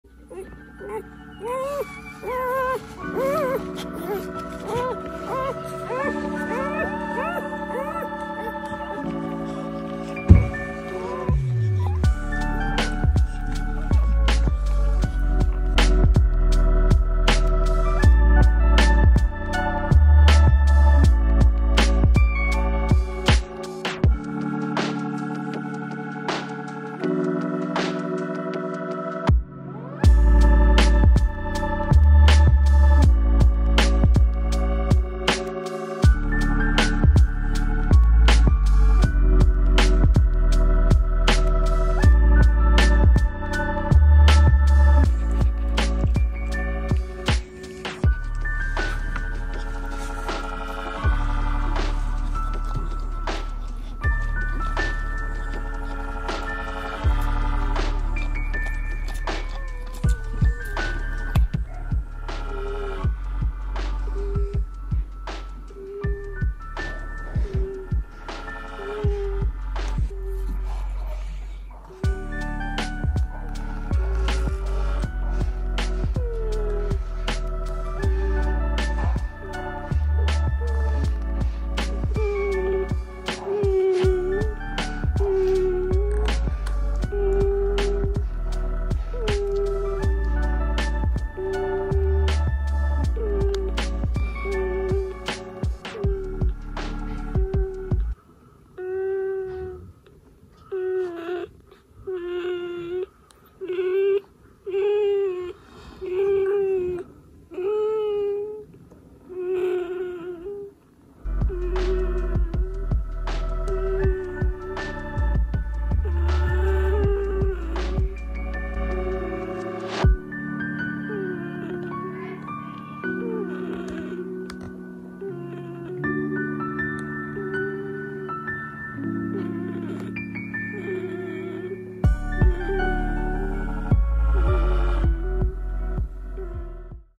The Cutest Puppies in the World | Newborn Samoyeds
What newborn Samoyed puppies look like. The cutest dogs in the world.
Samoyed breed.